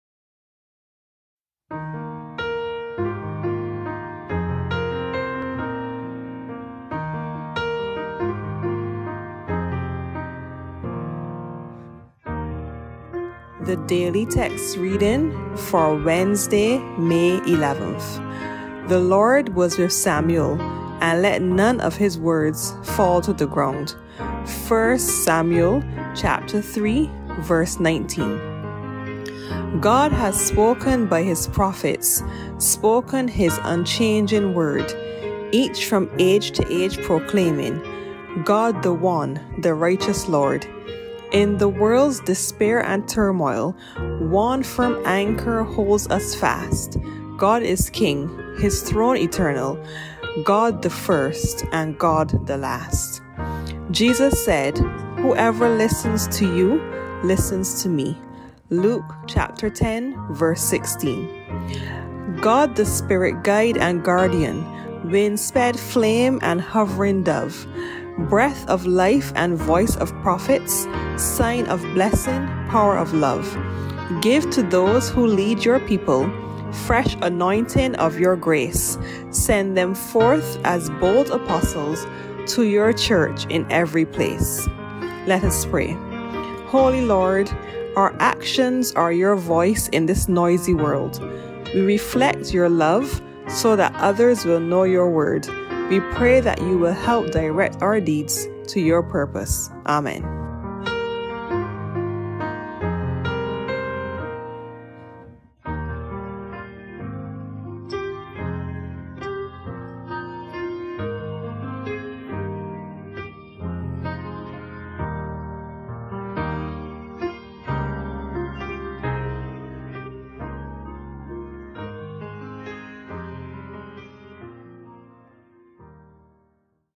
Moravian Daily Texts: Wednesday May 11, 2022
Today's reading is done by our youth member, Julienne St. Hill.

Celebrating Child Month!